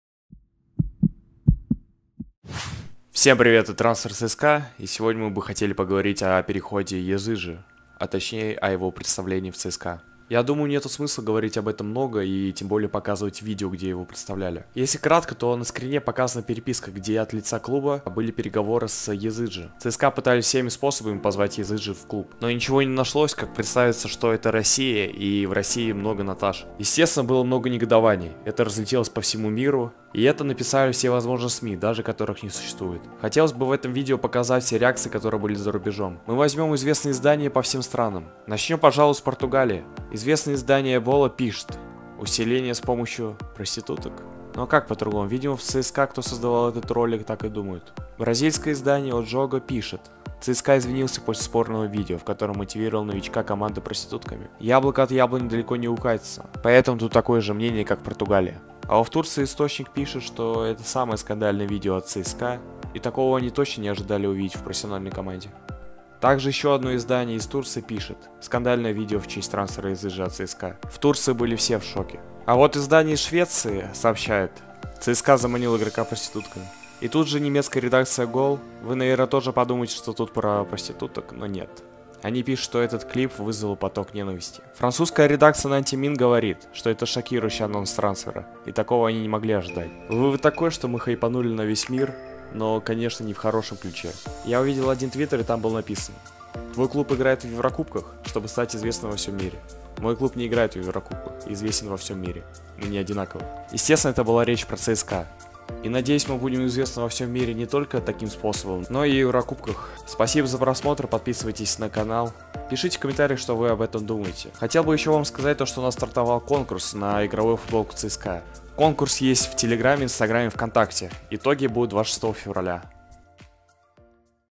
🇹🇷 РЕАКЦИЯ ЗА РУБЕЖОМ НА ЦСКА И «НАТАШ» | СЕНСАЦИЯ С ПЕРЕХОДОМ ЯЗЫДЖИ
🌐 Теги: ЦСКА. Новый игрок в ЦСКА. Новый игрок. Трансферы. ЦВБП. Языджи в ЦСКА. Трансферы ЦСКА. Футбол. Роналду. Месси. Футболисты.